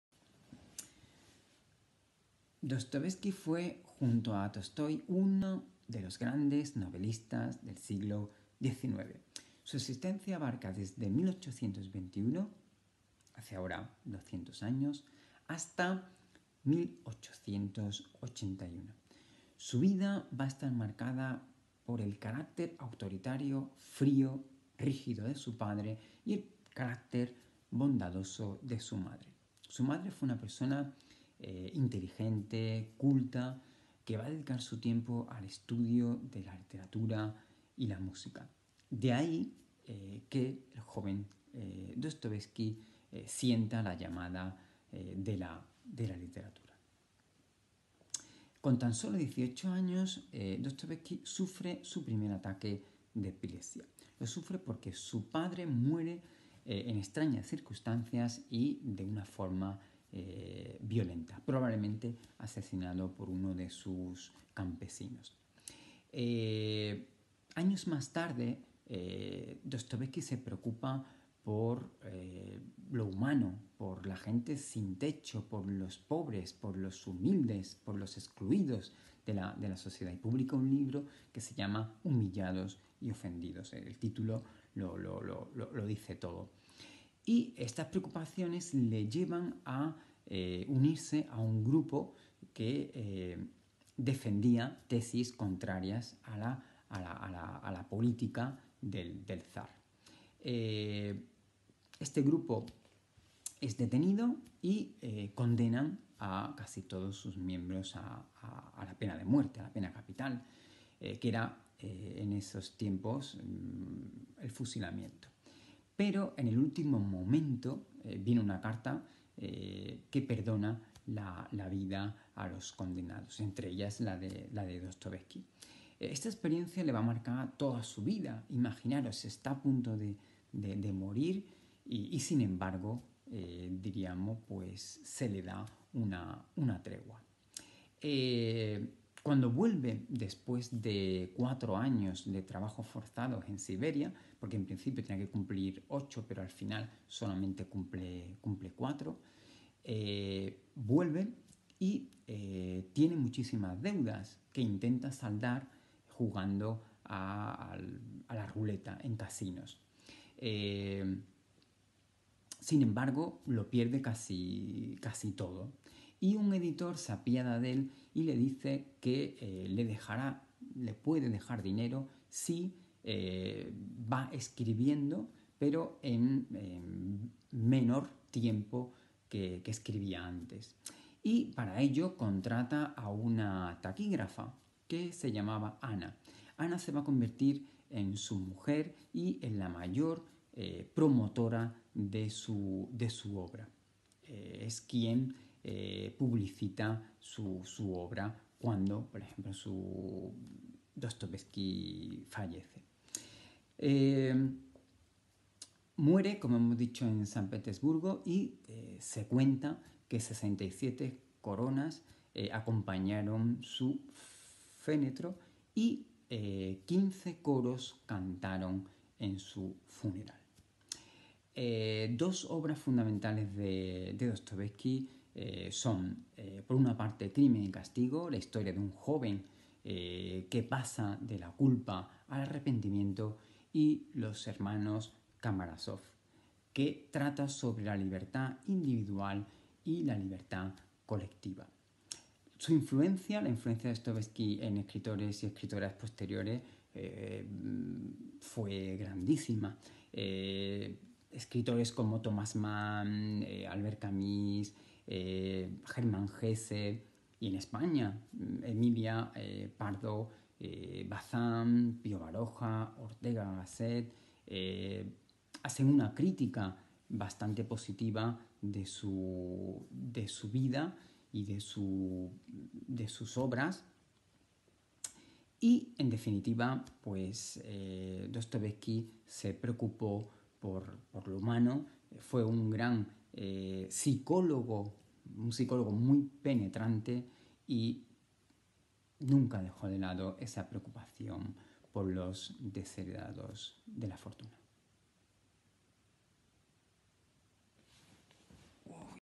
Conferencia sobre Dostiesvki.
Conferencia sobre Dostiesvki, a cargo de Francisco Jesús Ahumada Bellido, Profesor de IES Miguel de Cervantes Saavedra.

Video cedido por gentileza de la Asociación Callejón del Trapo de Sanlúcar de Barrameda.